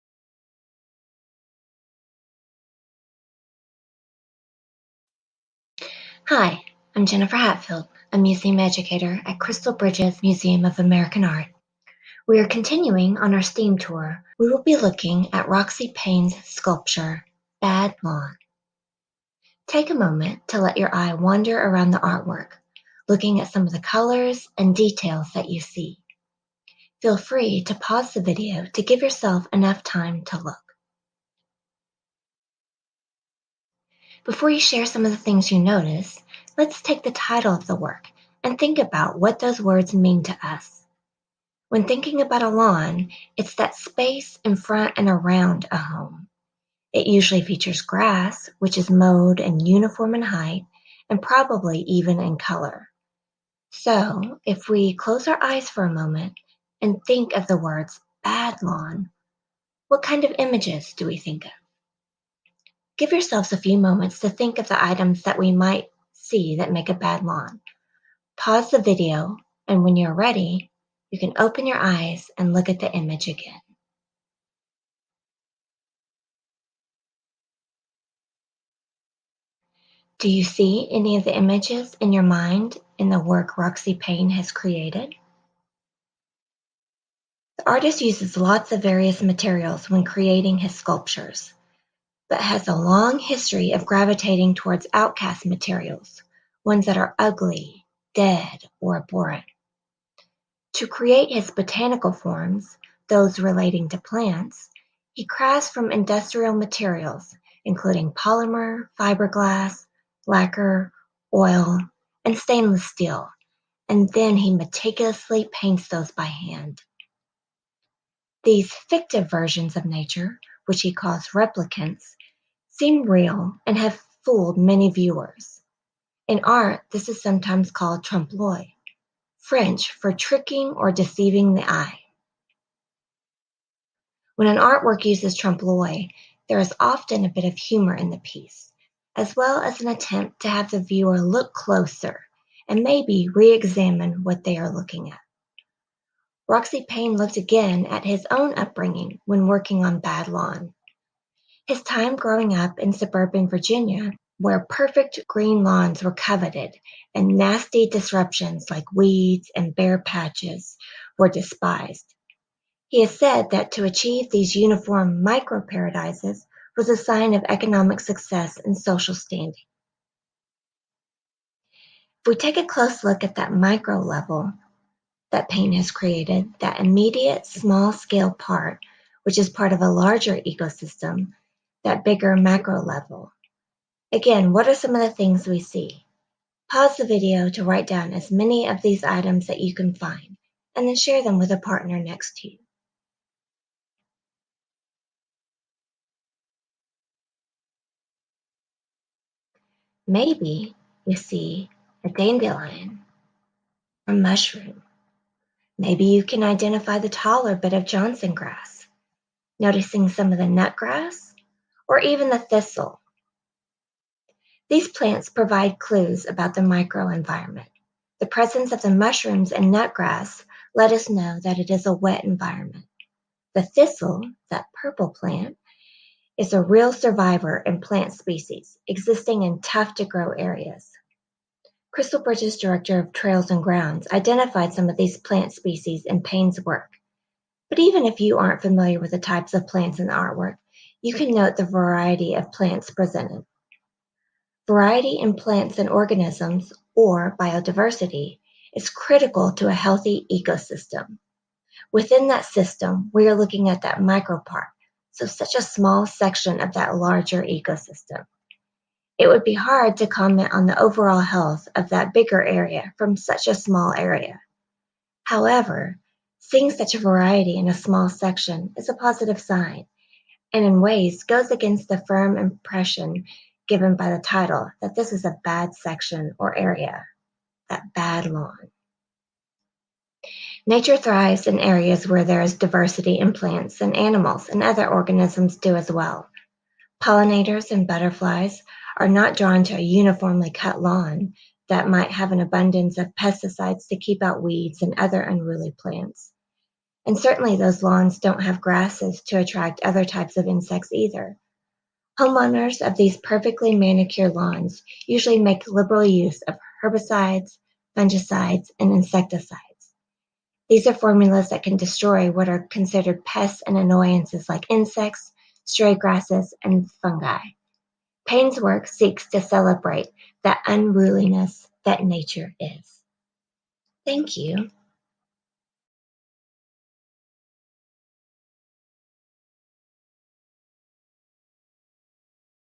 Roxy Paine's "Bad Lawn", 1998
Roxy Paine’s "Bad Lawn" gives viewers the chance to think about how the objects in the setting he created relate to one another, as well as the larger role that they play in our environment.